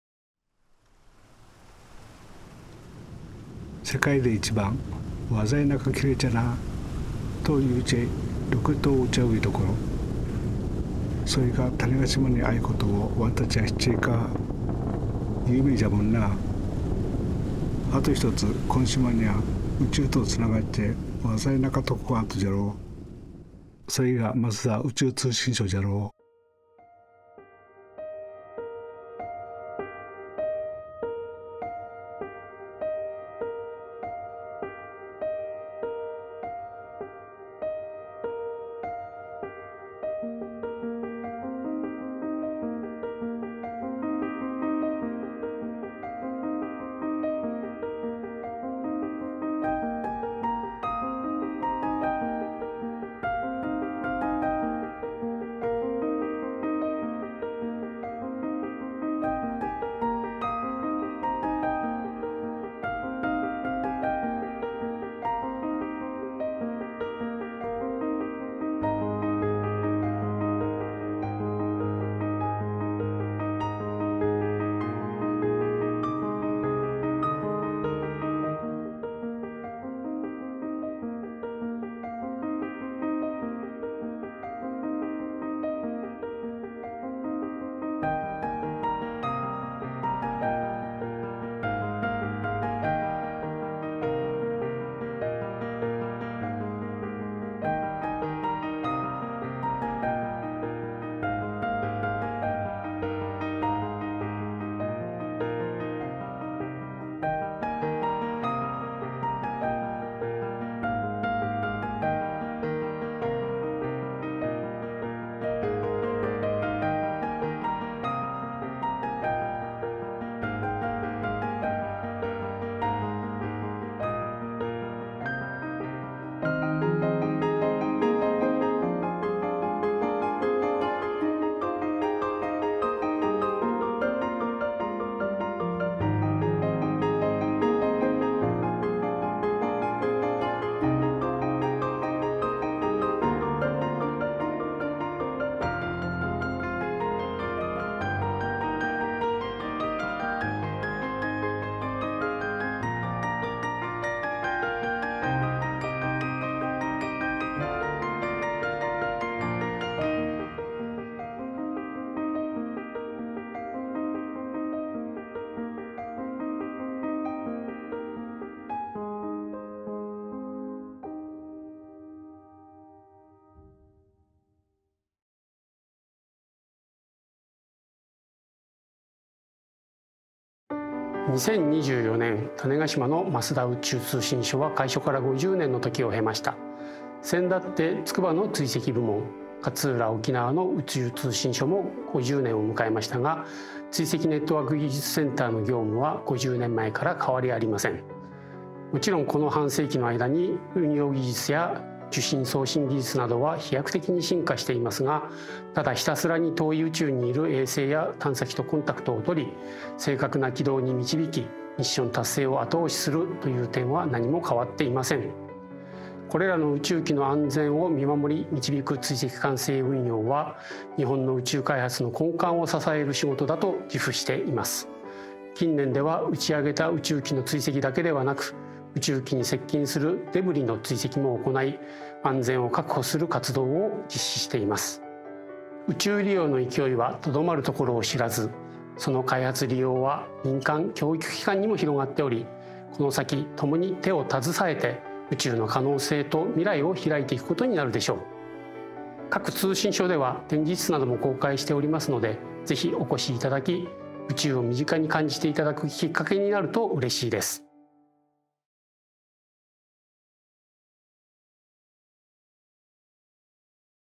増田宇宙通信所開所50周年記念動画「これまでも、これからも」
増田宇宙通信所は、2024年開所50年目を迎えました。
50年を経て技術は各段に進歩しましたが、昔も今も変わらず地上から宇宙を見守るという重要な任務を担っています。

▼JAXA（国立研究開発法人宇宙航空研究開発機構/Japan Aerospace Exploration Agency）